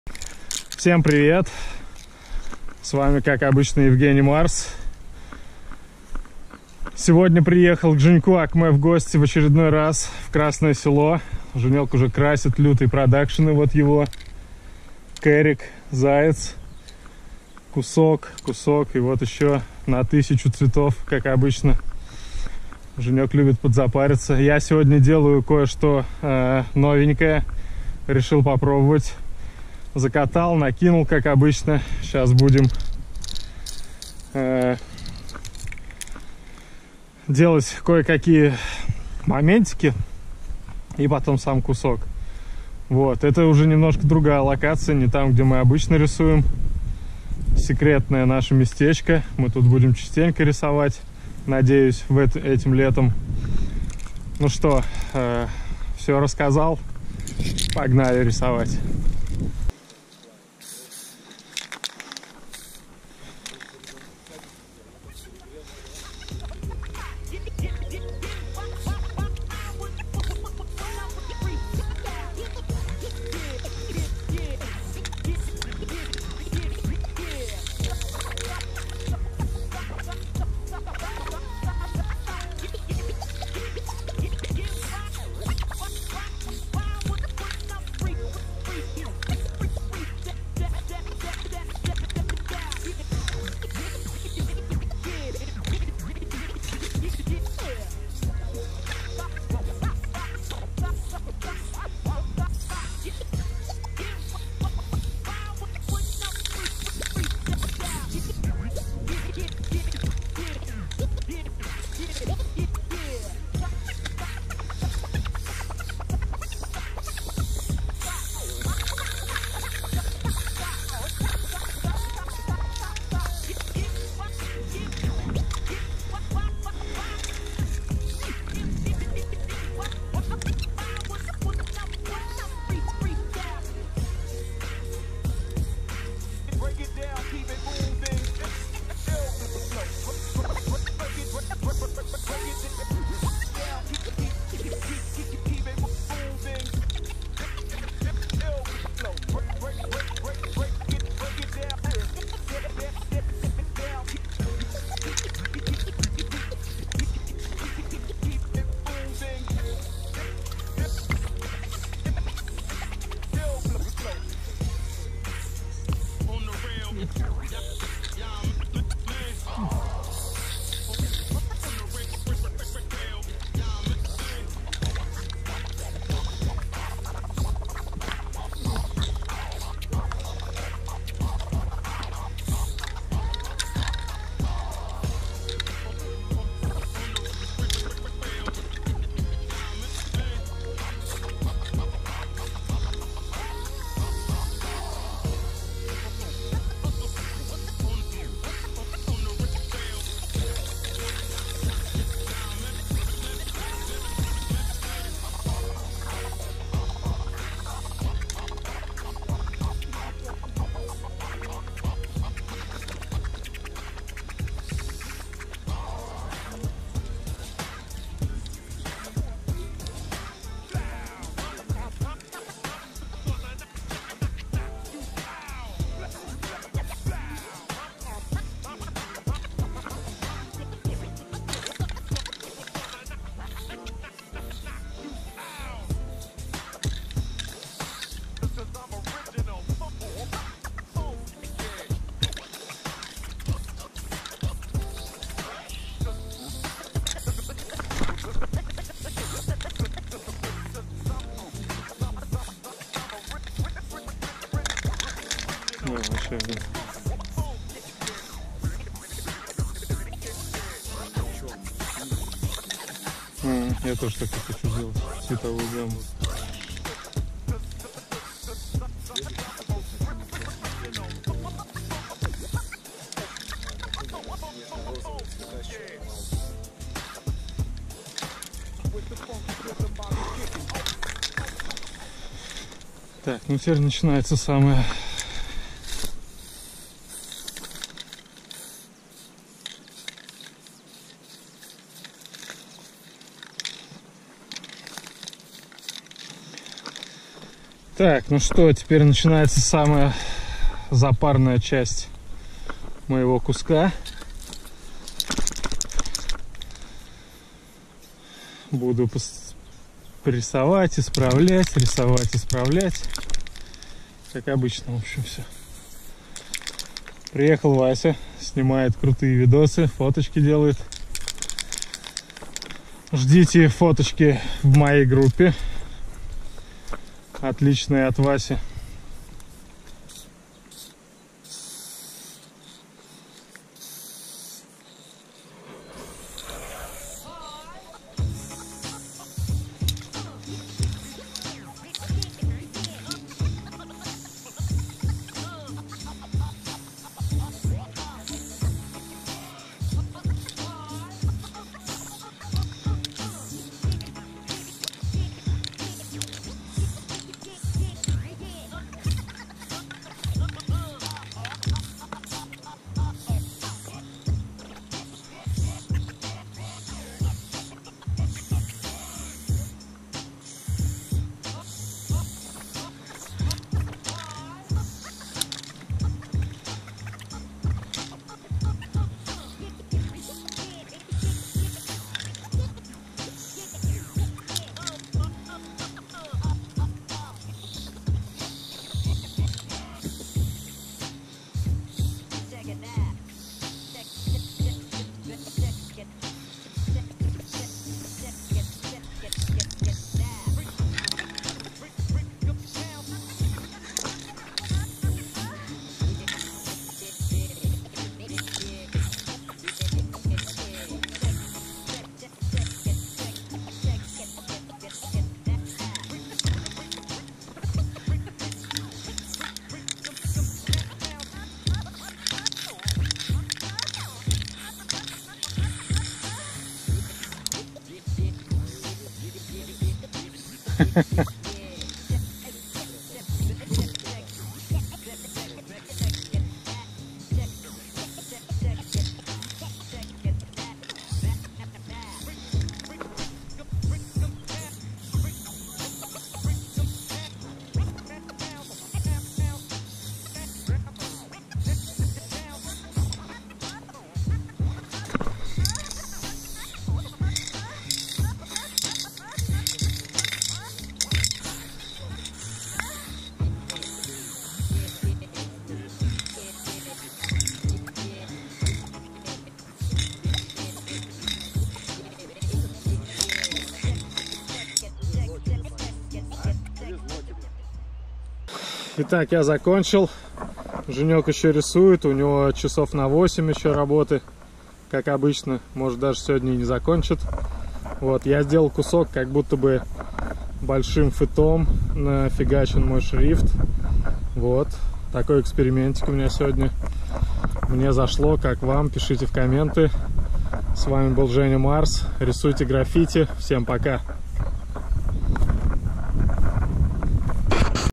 Граффити в стиле таги фет кепом / Fat cap tag graffiti style
Эксперементирую с разными стилями рисования своих шрифтов. В этот раз делаю буквы как теги фэтом в Красном Селе